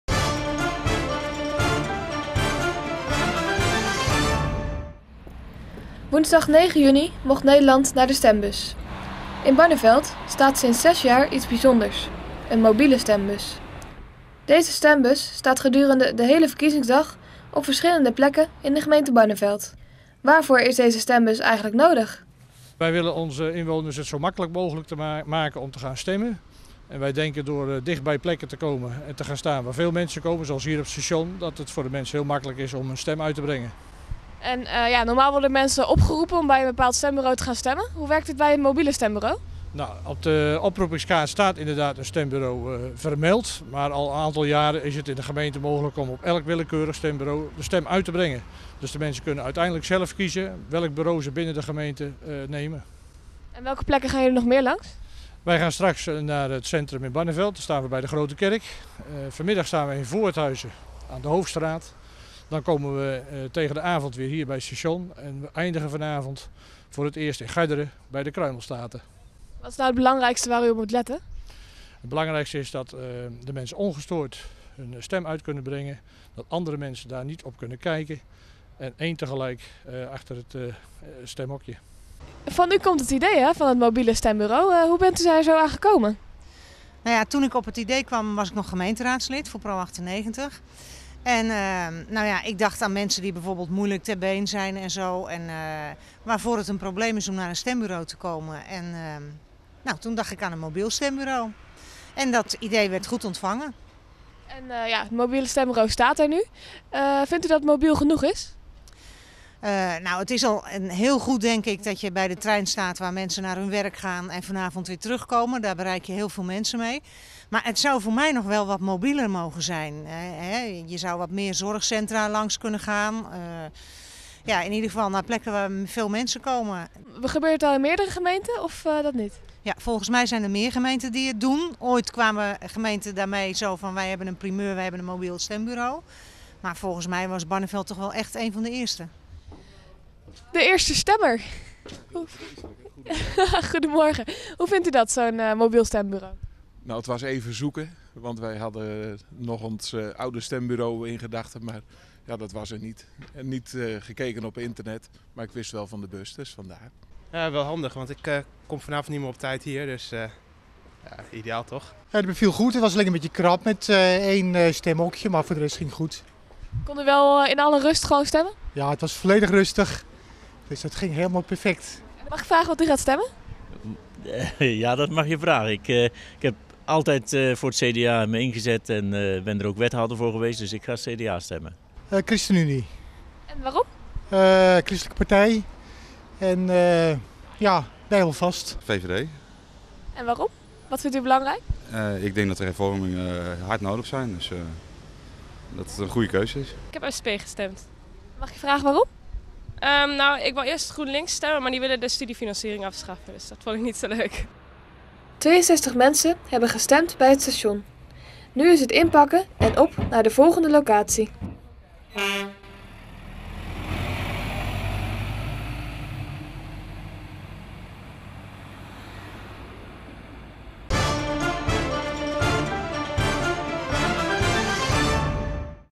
Barneveld stemt in mobiel stembureau
In Barneveld konden de stemgerechtigden op een bijzondere manier hun stem uitbrengen. Een mobiel stembureau stond op verschillende plaatsen in Barneveld.